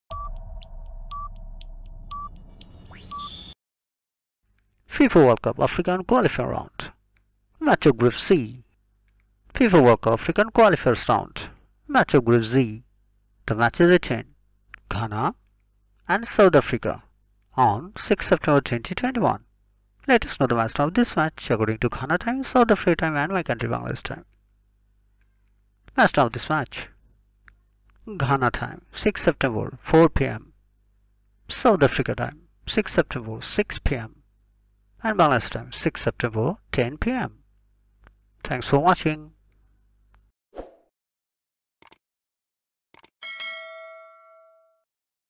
Ghana vs South African Football Match - 06 September 2021 - FIFA World Cup African Qualifiers
Winning the first match, Ghana is keeping the no. 1 position but drawing the first match South Africa is staying at no. 2 in this group.
Get this match time according to Ghana, South Africa and Bangladesh time.
Also check for the other matches schedule of this tournament in this Channel.